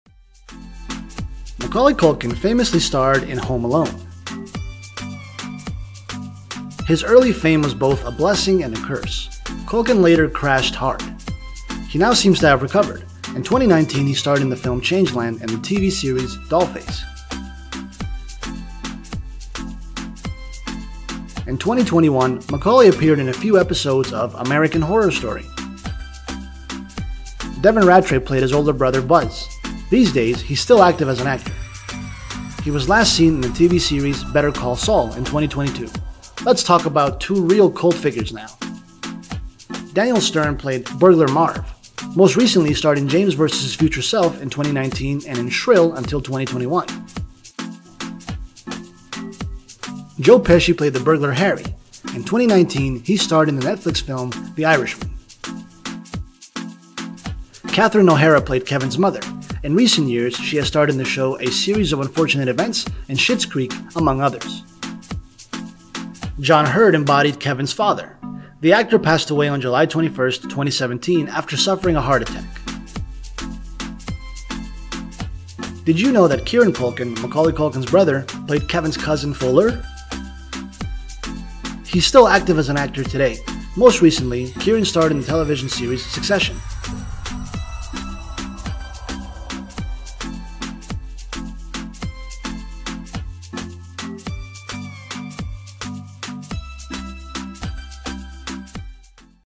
'Home Alone': The Cast Today | Celebrity Hot Goss
Home Alone is a family Christmas comedy that came out in early 1990.
Find out what the stars look like today!